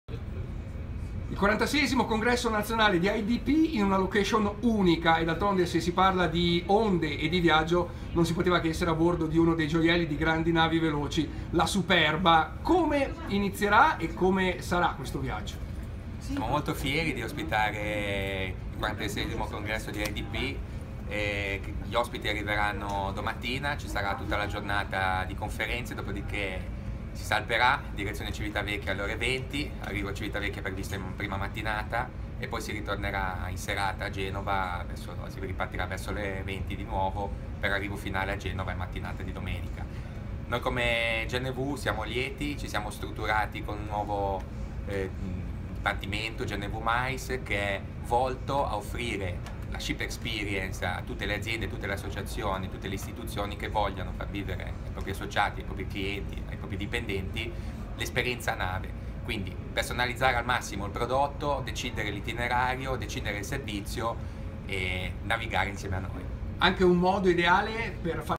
Il Congresso Aidp a bordo di GNV
Tante personalità e anche due campioni di sport come Maurizia Cacciatori e Alex Bellini